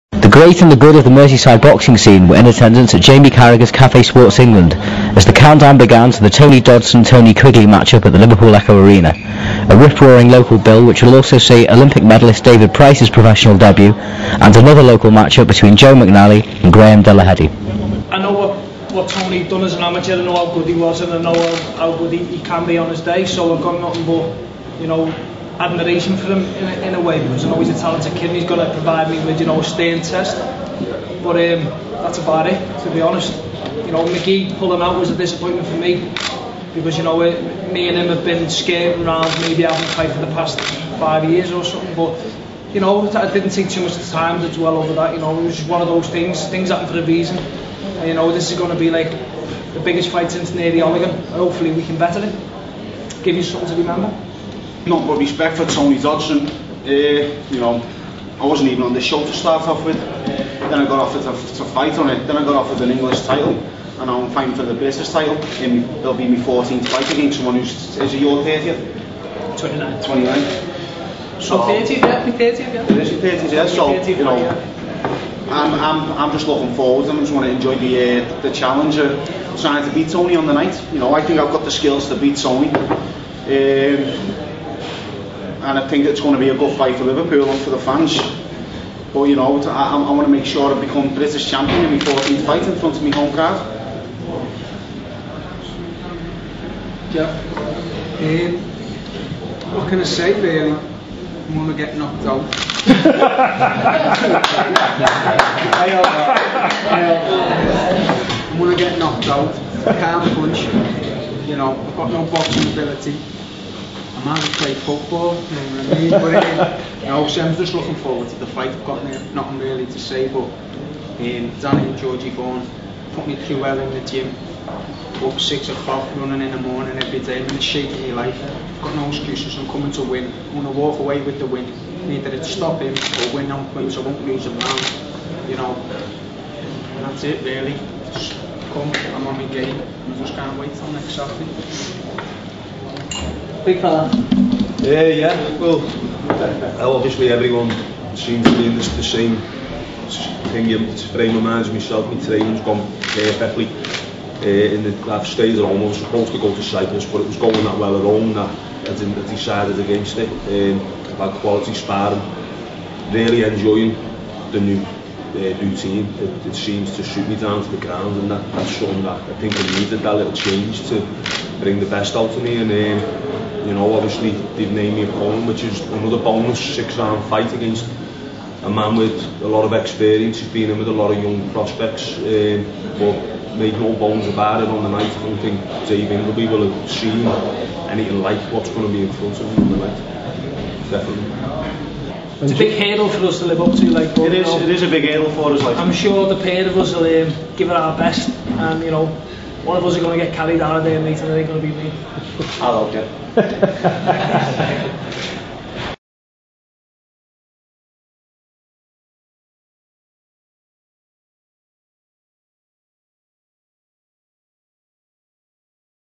Liverpool ECHO Arena set for 'Cock of the Dock' showdown
ONE of the biggest local fight nights in years is set to hit the ECHO Arena as Tony Dodson takes on Tony Quigley with Olympic champion David Price making his professional debut on the same Hayemaker Boxing bill - get tickets on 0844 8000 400